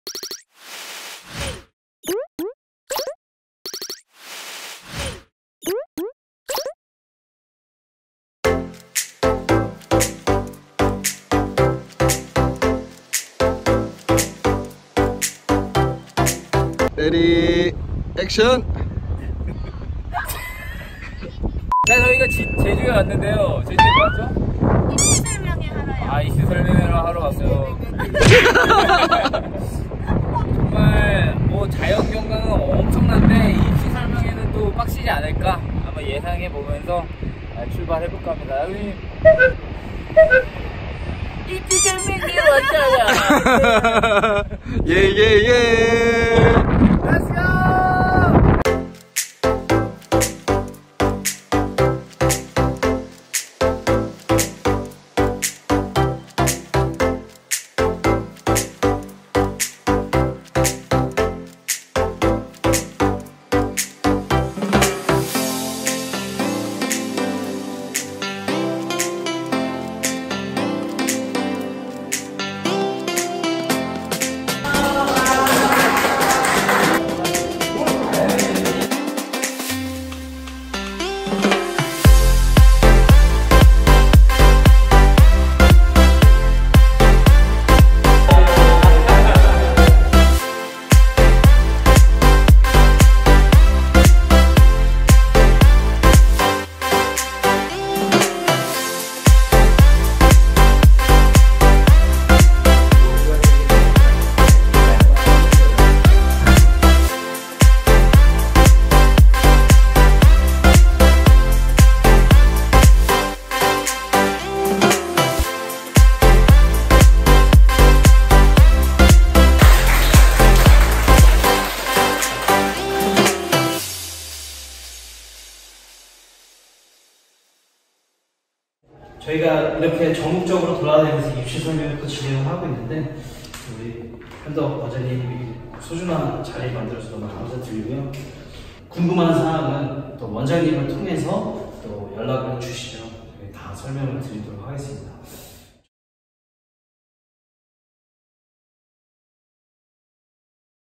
[HAC Sketch]2019 입시설명회 제주도편
전국투어 입시설명회
제주에도 역시 교수님들이 방문했씁니다.
전국 어디든 구분없이 학생이있다면
교수님들이 방문해서 여러분들께 많은정보 드리겠습니다.
언저든 연락주세요!!

학생들을 위한 학생들과 함께하는 즐거운 채널 실용무용계열.

실용무용계열 유튜브 구독 바로가기